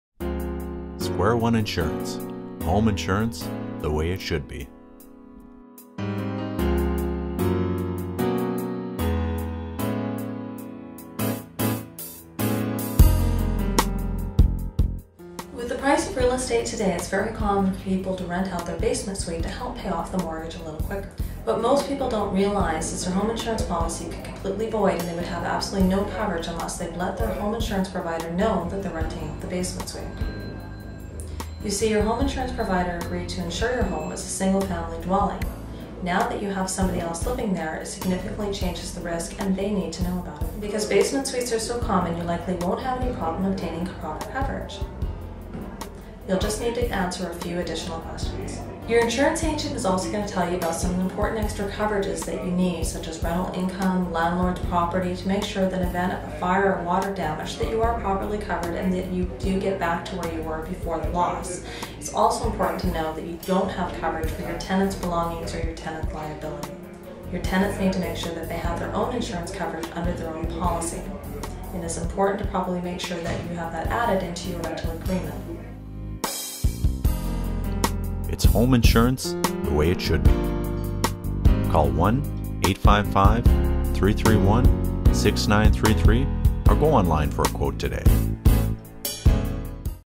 Renting Out a Basement Suite | Square One
So you've decided to rent out a basement suite to help pay the mortgage, do you know how this will impact your home insurance? Kris from Square One explains.

Square One offers personalized coverage to fit your individual needs.